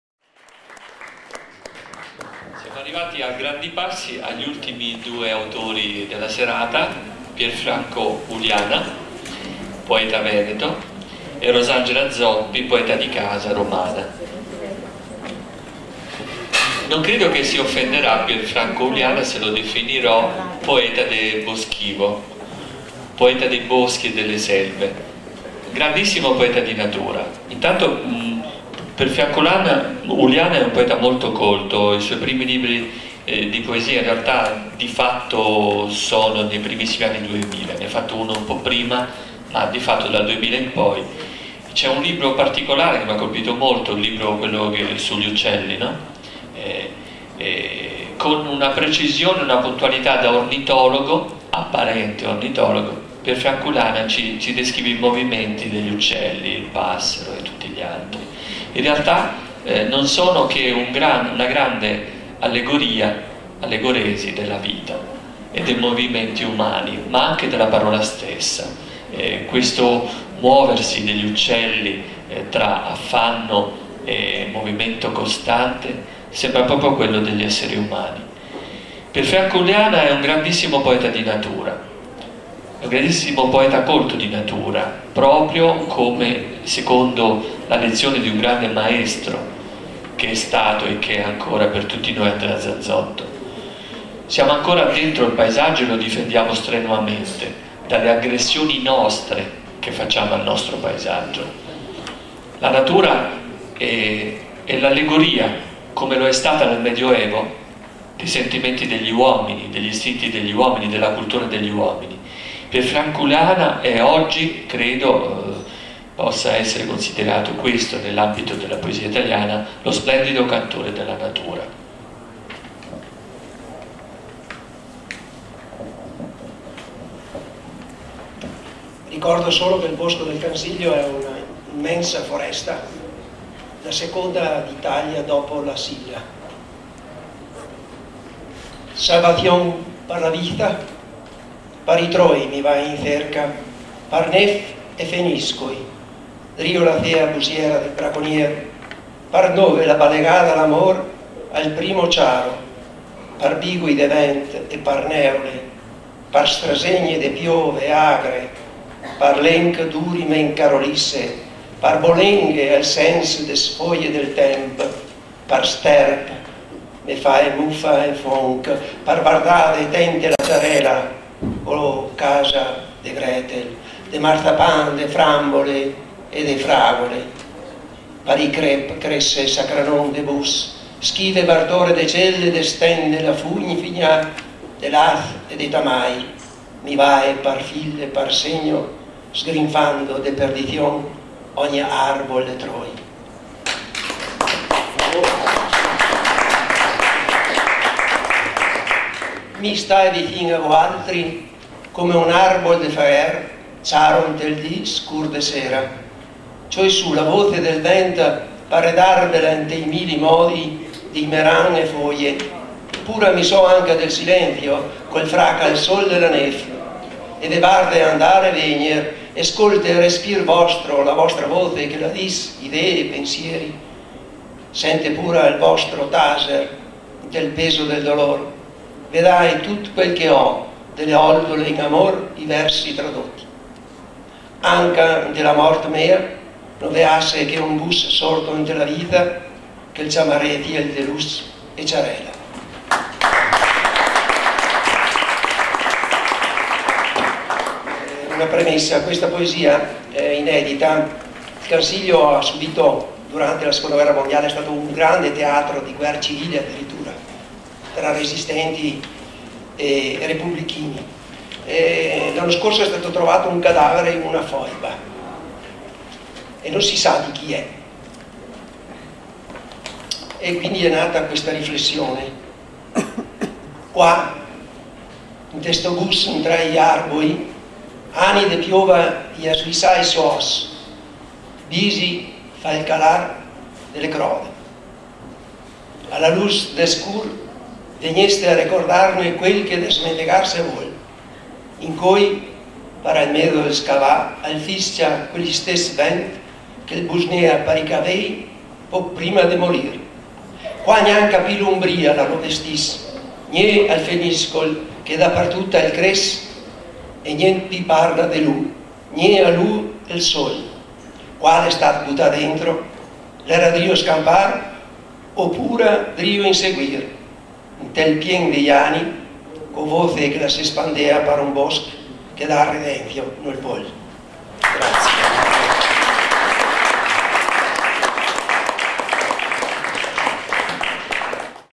Pier Franco Uliana Dialetto veneto del Bosco del Cansiglio - BL-PN-TV
ALTRE LINGUE - Achille Serrao
Biblioteca Nazionale Centrale di Roma
Reading dei poeti 22 Marzo 2019